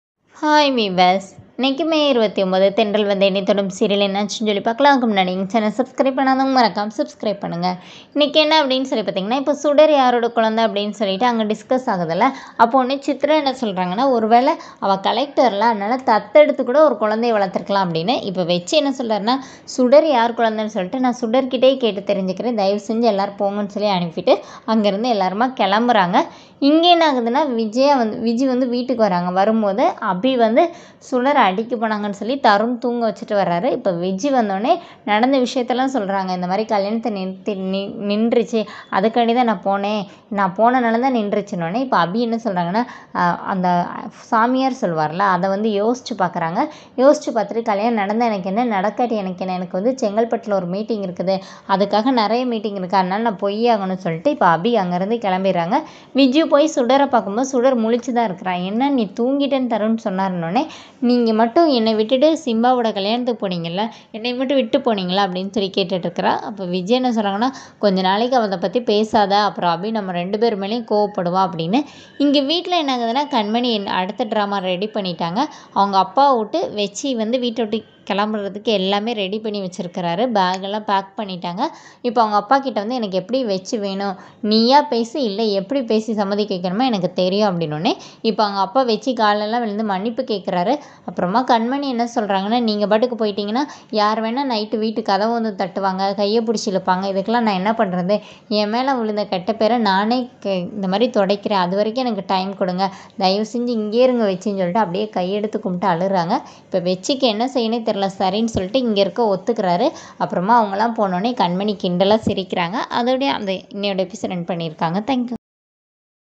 வெற்றி மனதை மாற்றும் கண்மனி||TVET Review||29 May
அபியிடம் உண்மையை சொல்லும் விஜி.